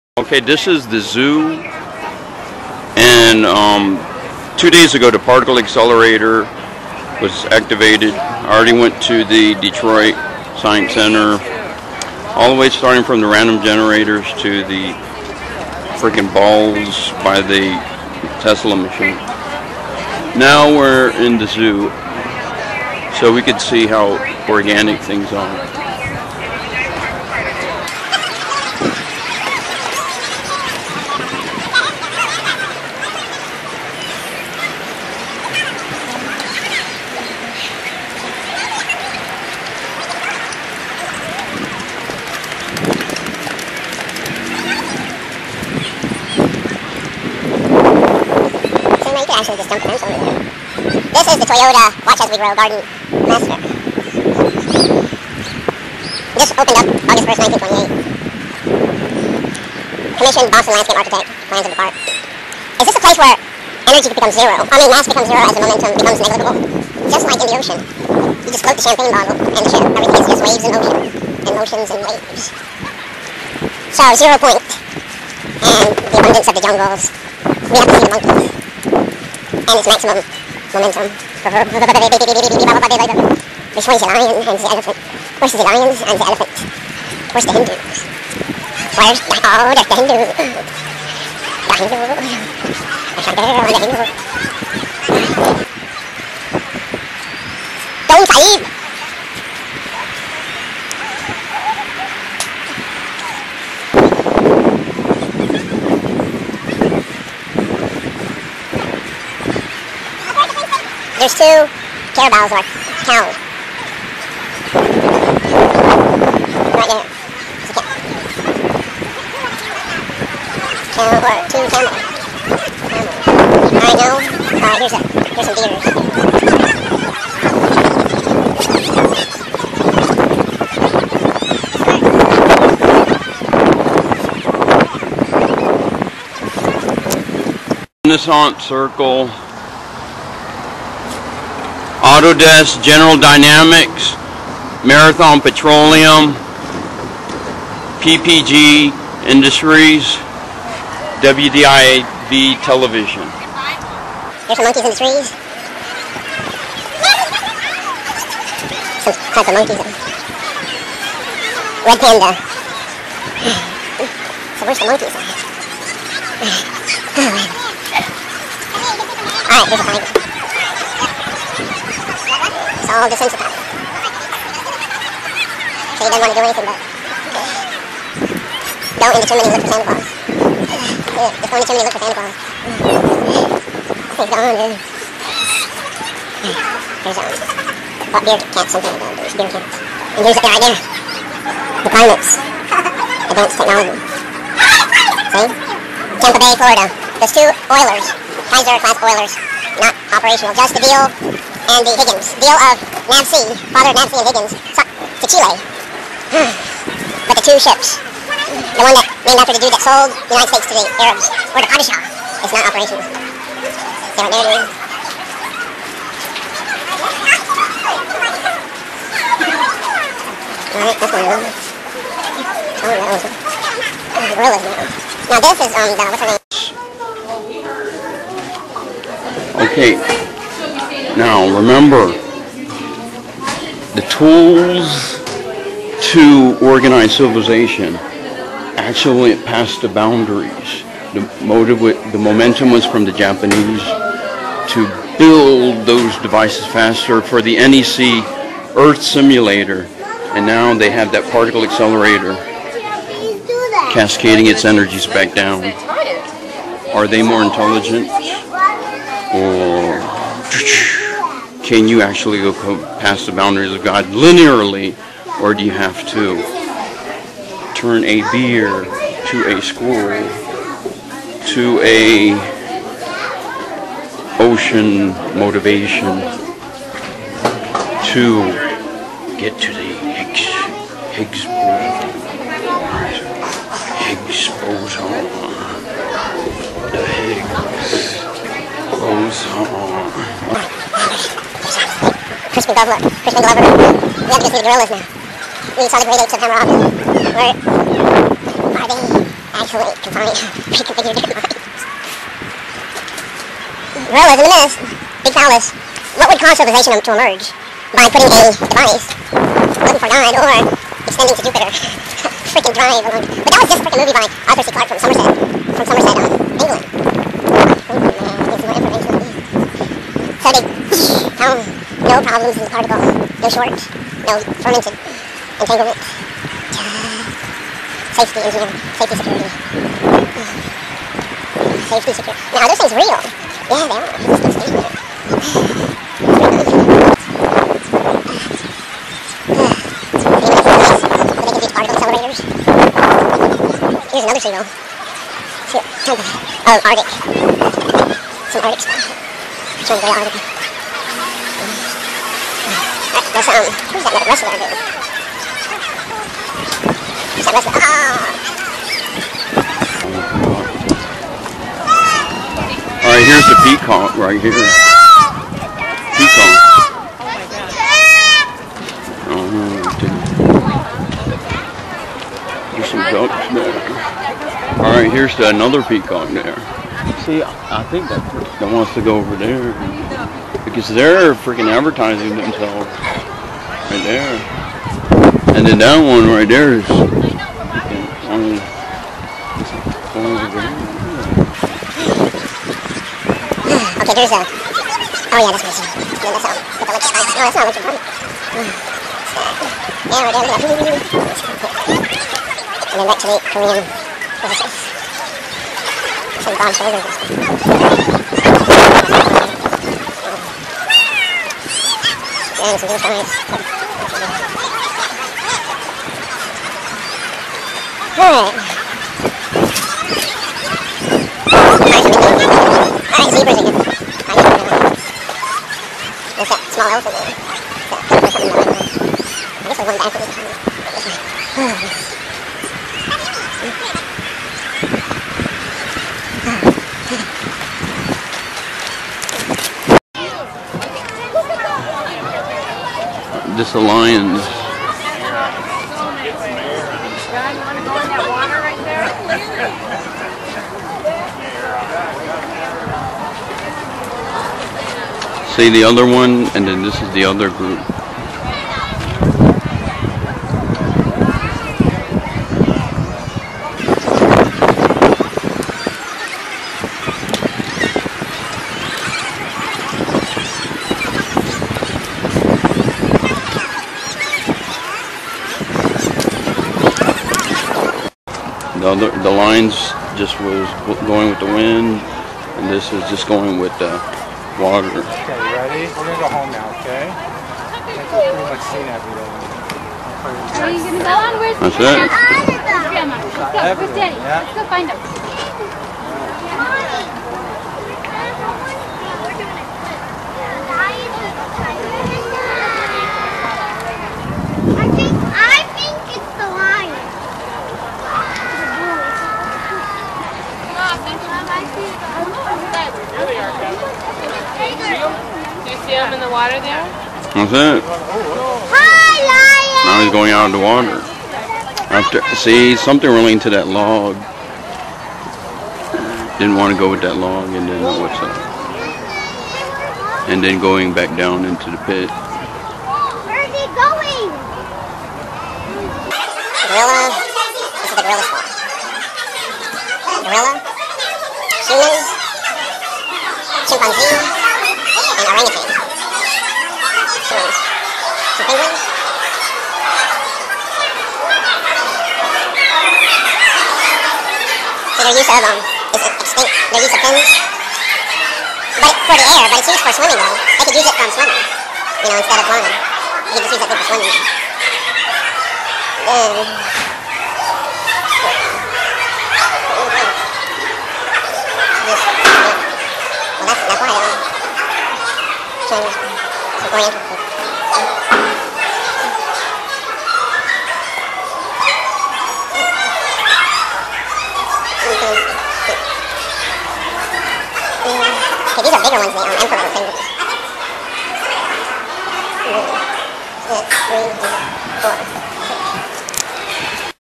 Detroit Zoo after Science Center Fermilab professor lecture and Large Hadron collision
The Detroit Zoological Park, commonly known as the Detroit Zoo, is located about 2 miles (3.2 km) north of the Detroit city limits at the intersection of Woodward Avenue, 10 Mile Road, and Interstate 696 in Royal Oak and Huntington Woods, Michigan, USA. The Detroit Zoological Society, a non-profit organization, operates both the Detroit Zoo and the Belle Isle Nature Zoo, located in the city of Detroit. The Detroit Zoological Society is responsible for the care and feeding of more than 1,800 vertebrates and 5,000 invertebrates representing over 270 species.

The first Detroit Zoo opened in 1883 on Michigan and Trumbull Avenues, across from the then site of Tiger Stadium. A circus had arrived in town, only to go broke financially. Luther Beecher, a leading Detroit citizen and capitalist, financed the purchase of the circus animals and erected a building for their display called the Detroit Zoological Garden. The zoo closed the following year and the building converted into a horse auction.[2] The Detroit Zoological Society was founded in 1911, but the zoo's official opening did not occur until August 1, 1928.